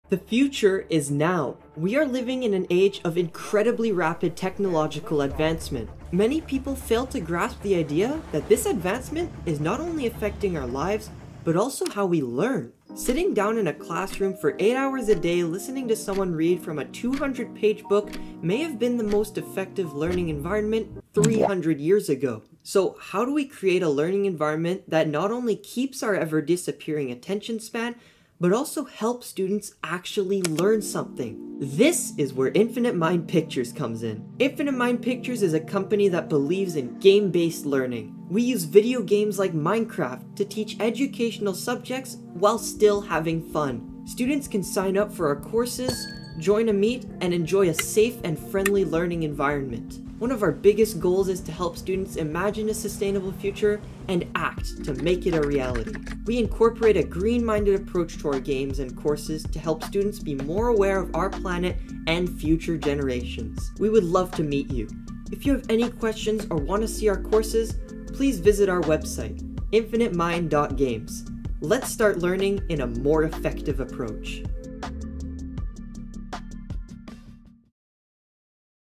About us - Infinite Mind Pictures
Infinite Mind Pictures is a company that believes in game based learning.  We use online games to teach kids educational subjects - while still having fun.

Learn more about us below